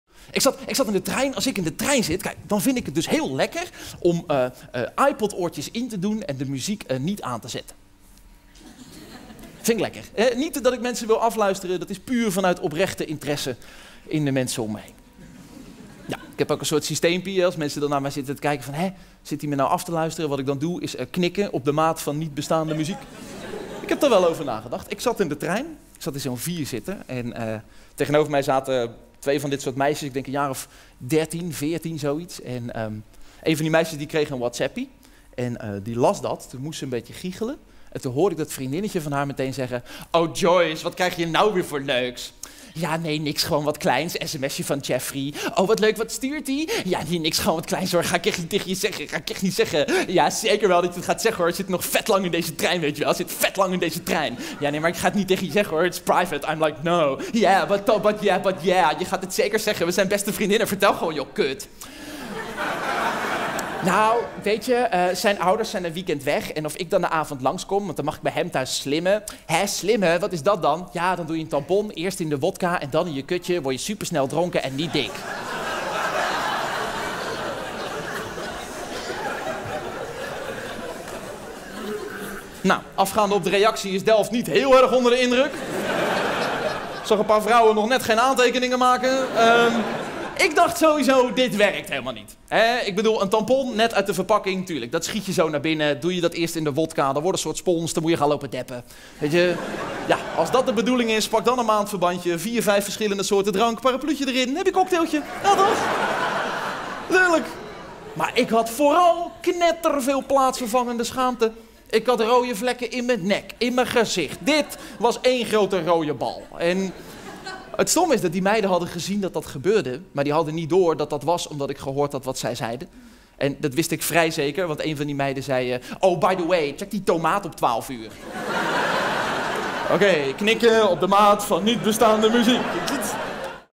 René van Meurs - Meisjes in de trein
René van Meurs is stand-up comedian bij Comedytrain en won in 2012 zowel de jury- als publieksprijs van het grootste cabaretfestival van Nederland; Cameretten.

Van september 2013 tot en met mei 2015 tourde hij langs de Nederlandse theaters met zijn debuutprogramma "Voor de storm". Dit is daar een fragment uit.

Opgenomen in Theater de Veste, Delft (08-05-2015)
Camera en montage: QMedia.nu
Theatertechniek: RR-Works.nl